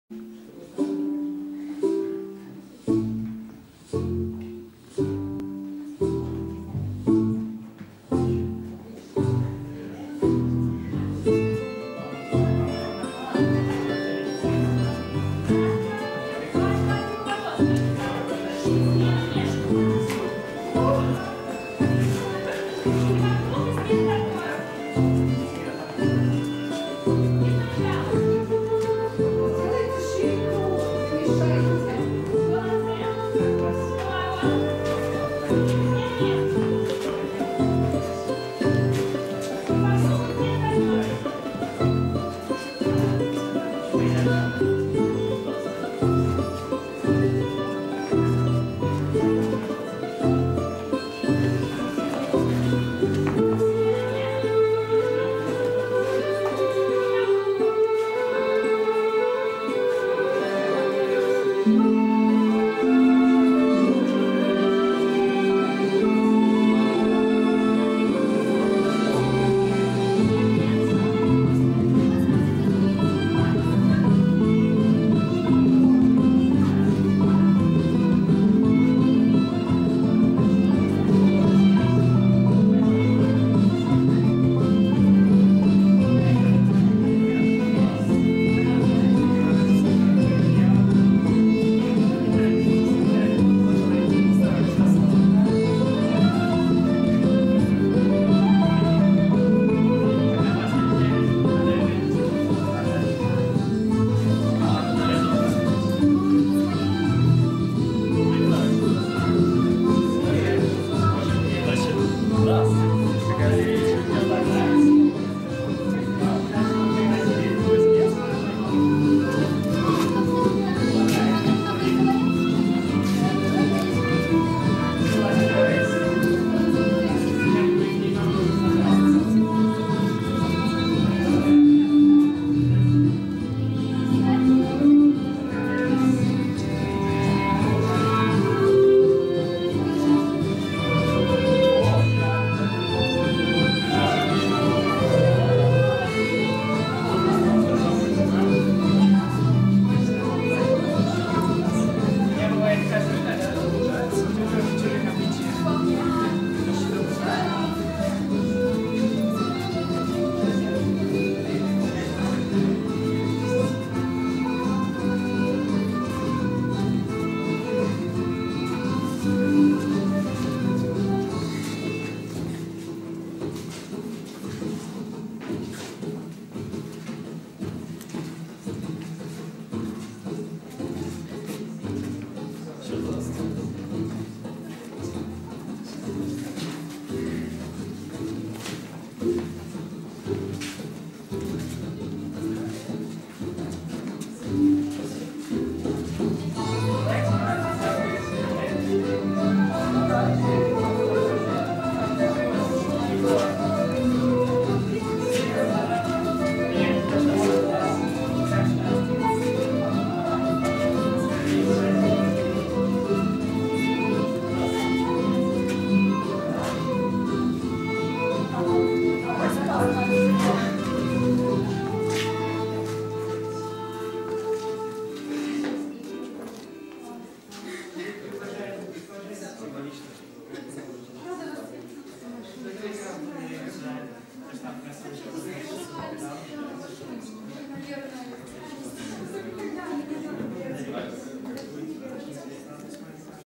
Вальс знакомств
Вальс знакомств со зрителями на студенческом театрализованном балу "Тайна средневекового замка" (ИФТИС МПГУ).